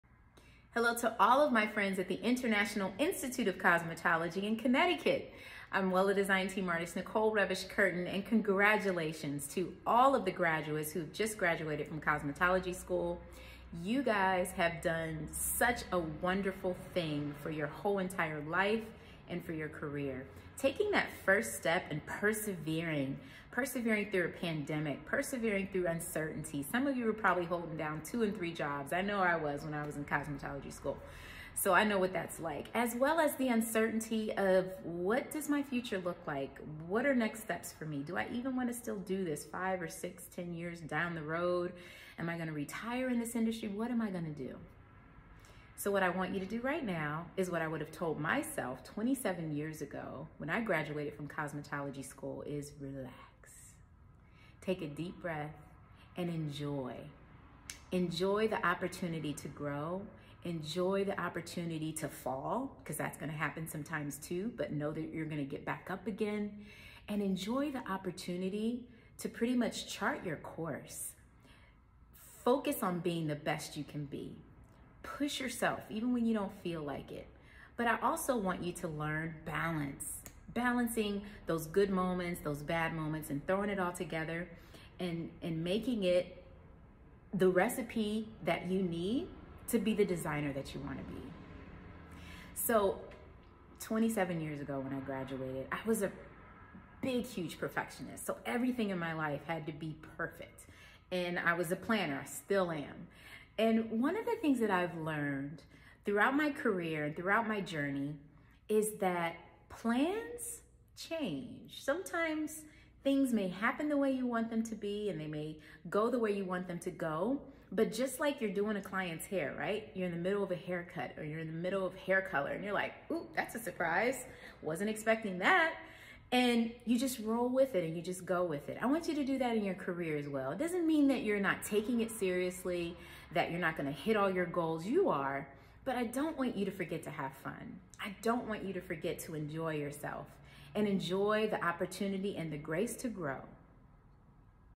Enjoy the Opportunities!
Wella Design Team Artist, Nicole Revish @nicolerevish, talks to International Institute of Cosmetology's graduating class about enjoying the opportunities that present themselves.